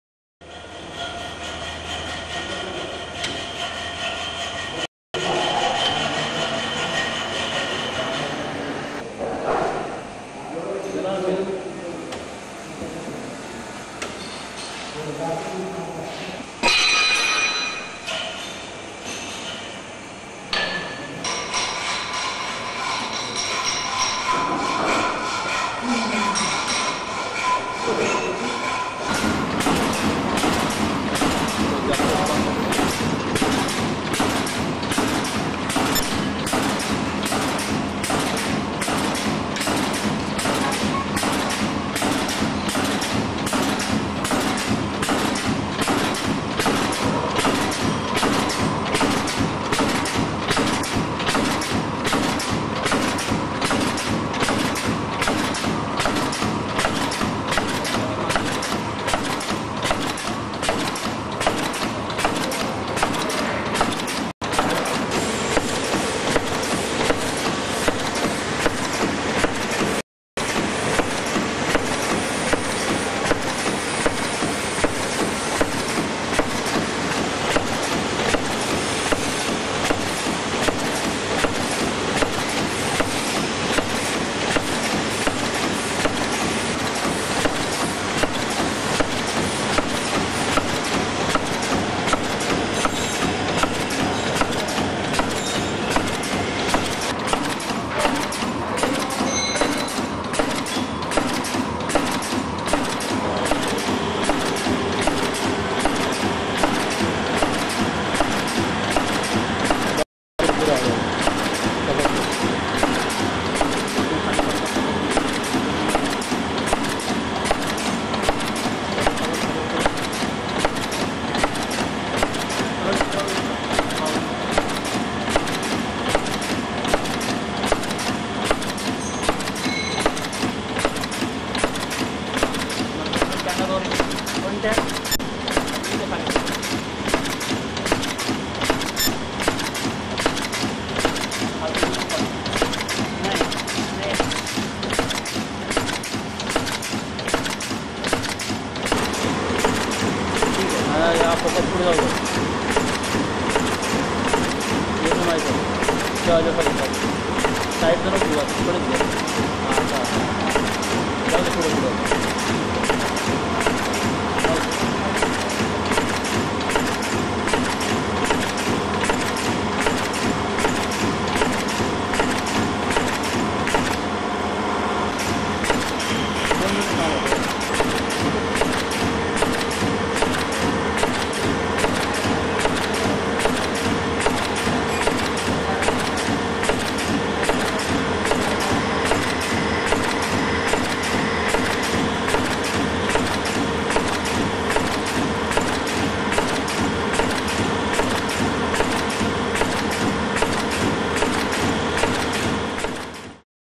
Multi track Sachets Packing Machine
-200 to 280 pouches/sachets per minutes
- useful to pack hair oil, shampoo, sauce, powders, dairy whiteners etc
- Reliable, low maintenance, easy to use